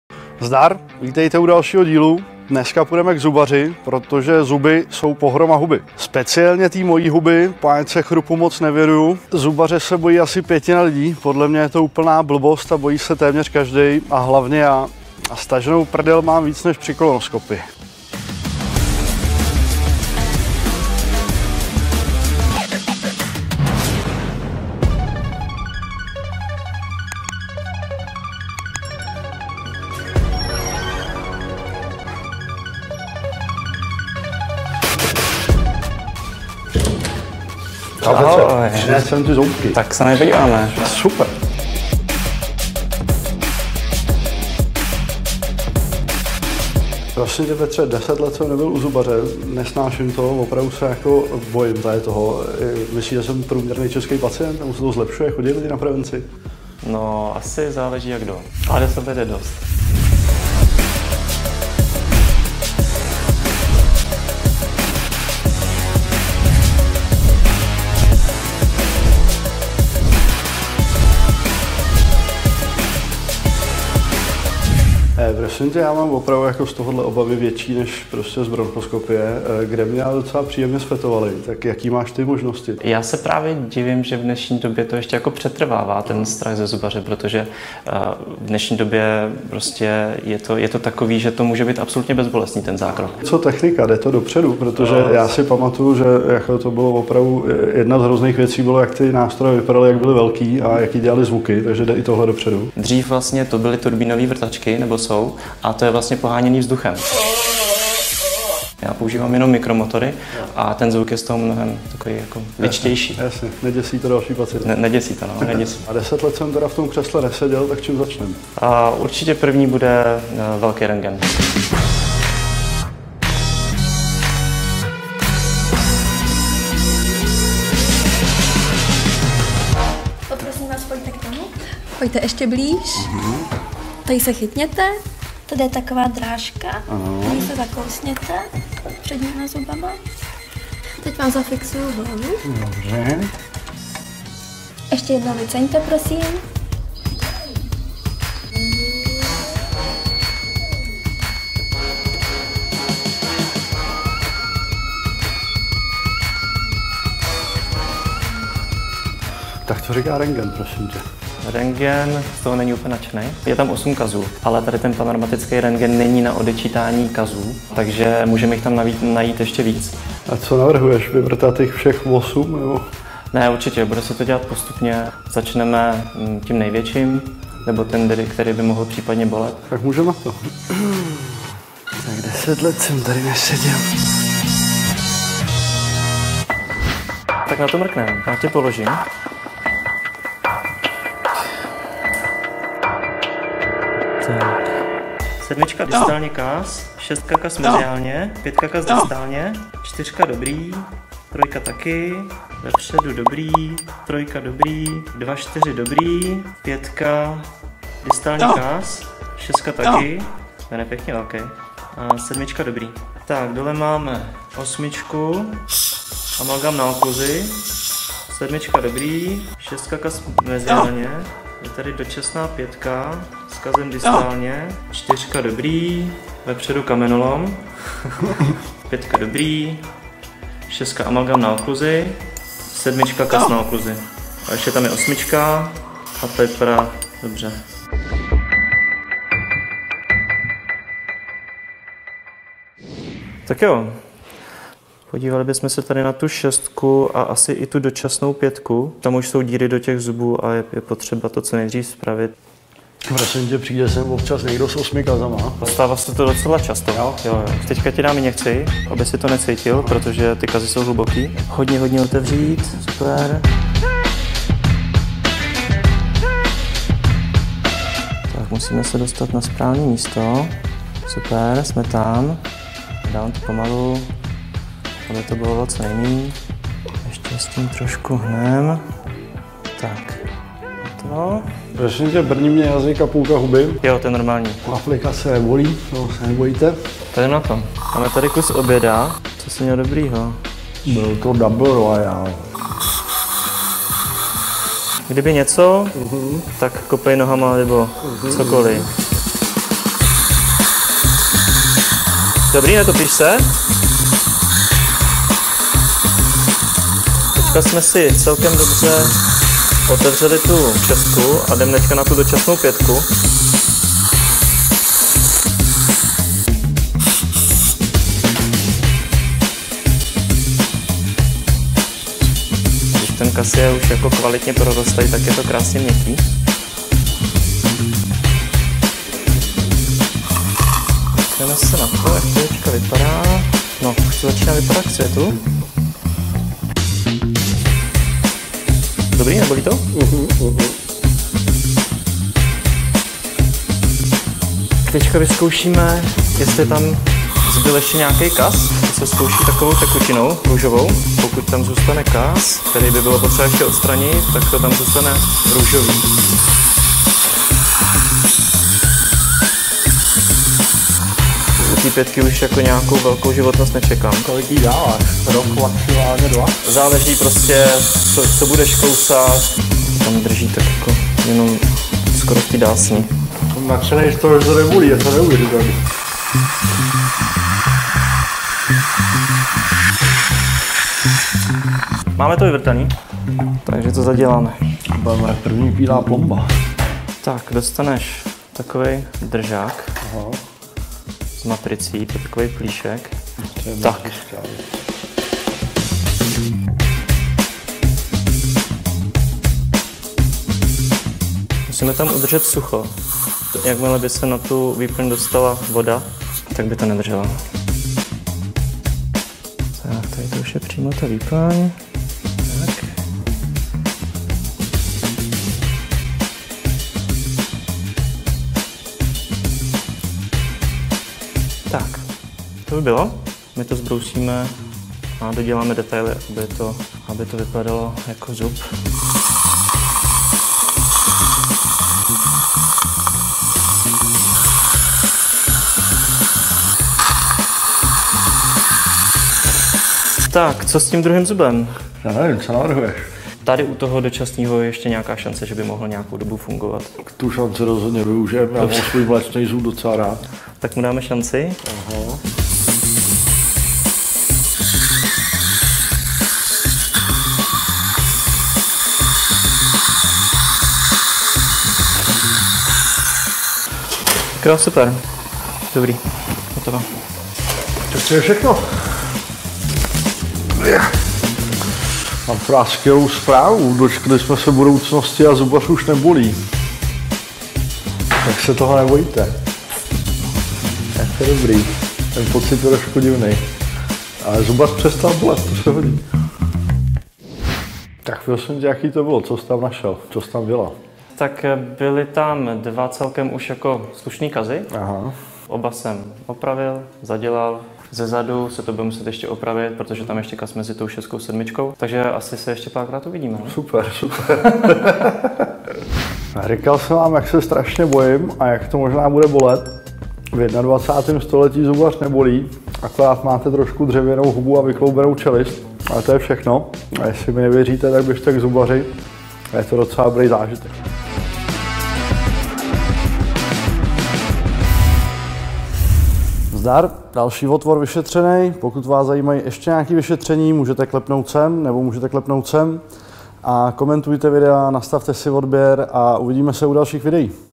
10 let nebyl u zubaře. Co má v puse?!
Rozklikněte pro víc informací 👇

Vít Samek nebyl 10 let u zubaře a bojí se ho jako čert kříže. Strach ale podle zubařů není na místě. Opravdu? Zubní prohlídka NA VLASTNÍ OTVOR!

Máte dotaz nebo se chcete s ostatními podělil o svou vlastní zkušenost s vyšetřením? Napište dolů do komentářů 👇